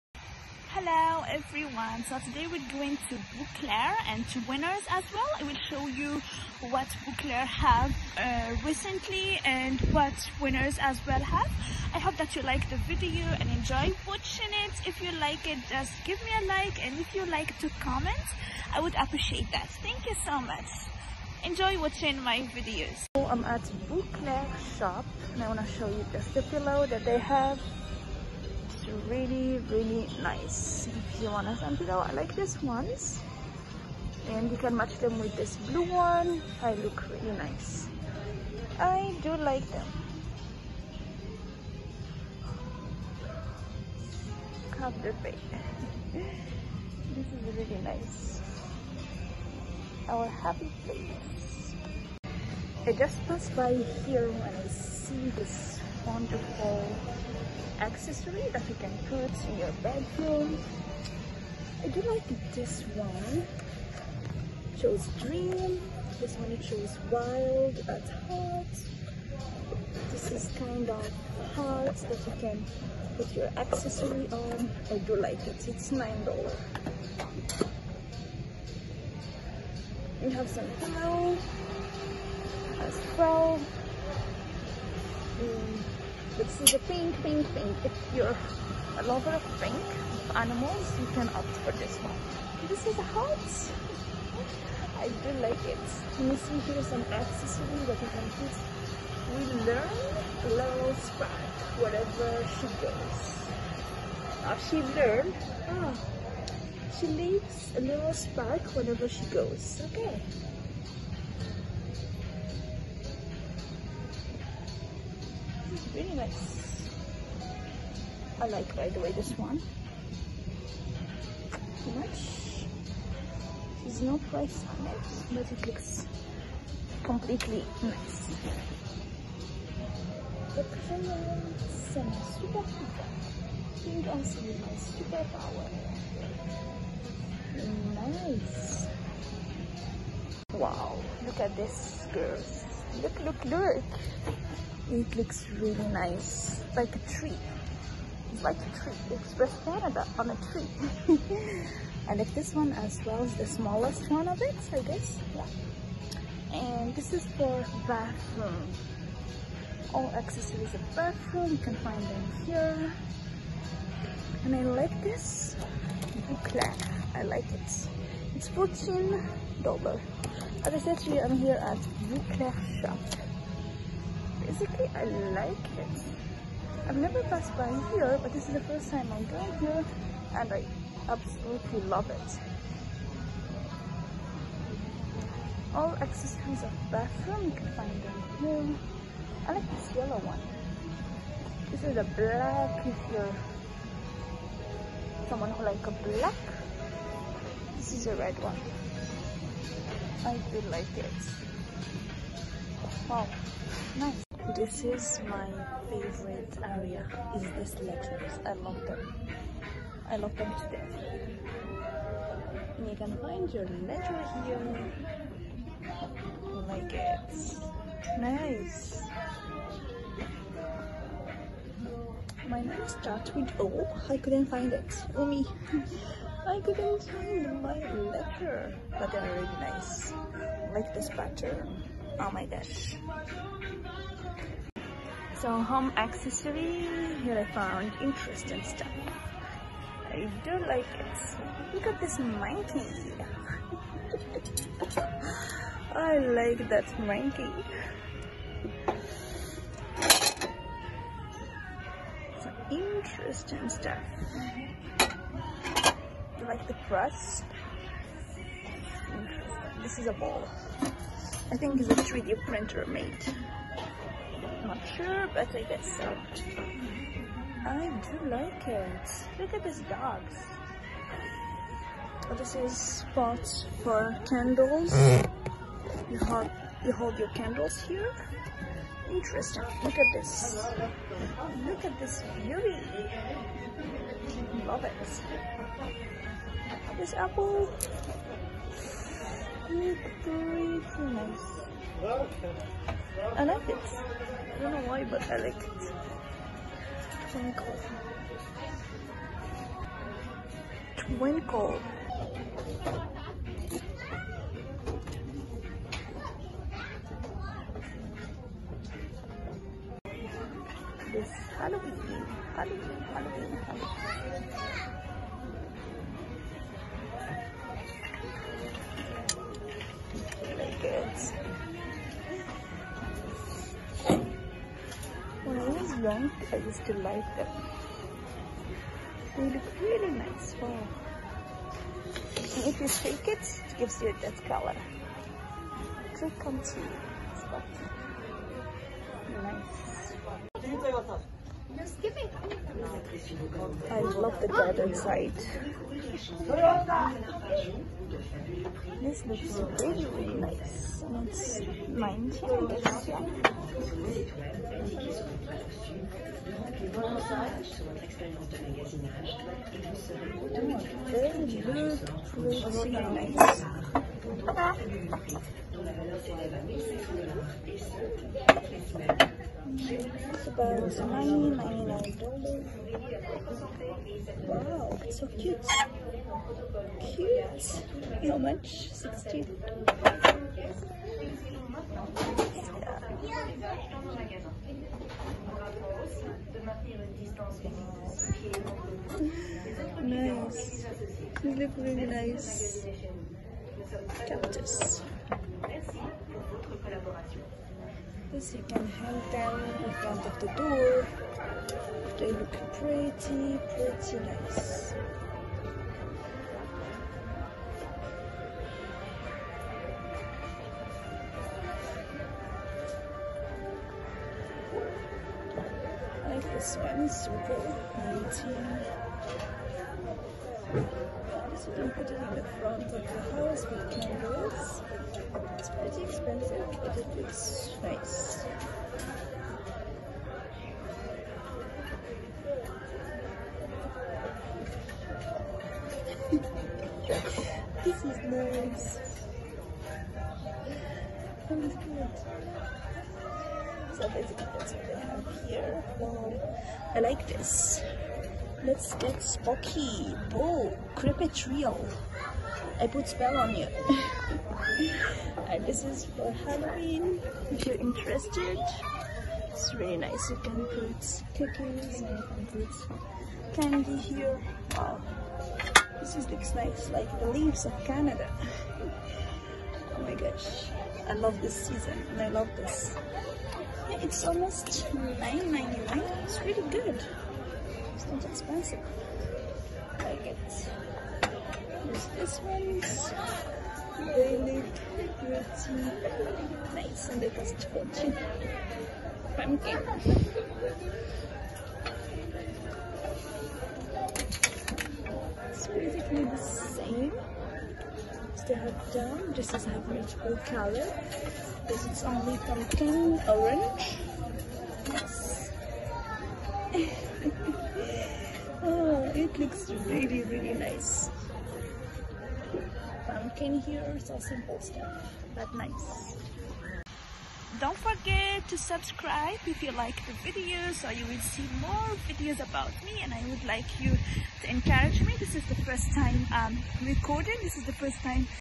Shopping with me at Bouclair & Winners
Hello Everyone,  Shopping video at Bouclair and Winners Montreal, I'm showing you all the latest product these two shops hold on their shelves.